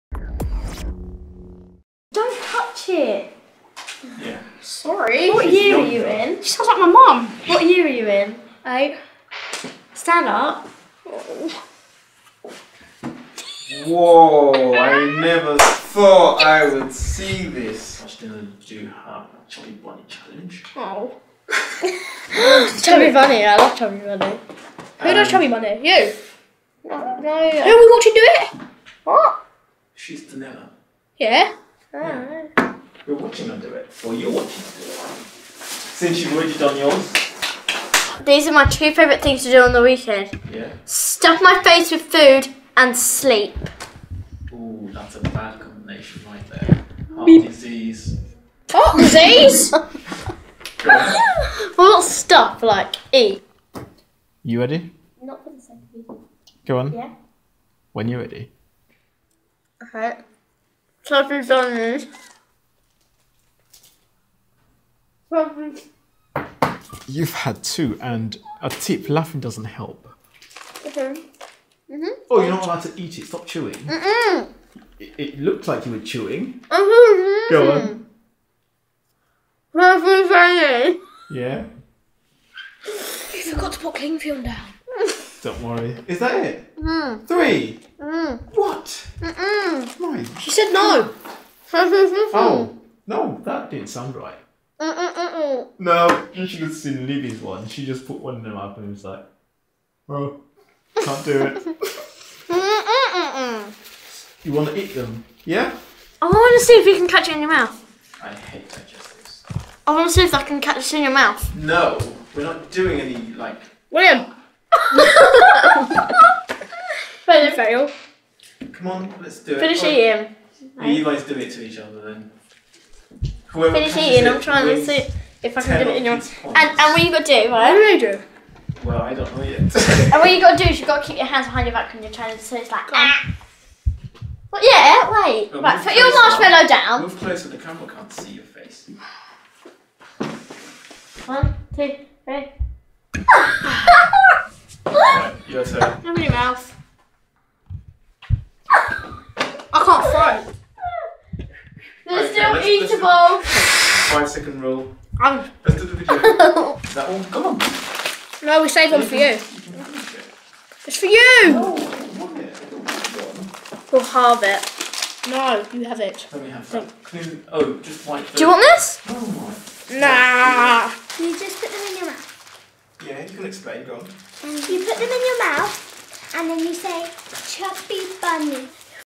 O Challenges! | Chubby Bunny #5 with Danella feat. Sonnyboy | ocUKids
More Chubby bunny challenge videos coming up XD
Facebook Twitter @OCUBOX ocukids. Suggest a video ideas/channel in the comments.
After all the chubby bunny challenge videos are up, we will be able to tell who had the most marshmellow in their mouth and were still able to say Chubby Bunny lol

Can you guess who the winner is already?. . .comment it below.
ALL OCUKIDS EPISODES
CHUBBY Bunny #5 with
Danella[11], & Sonnyboy[12].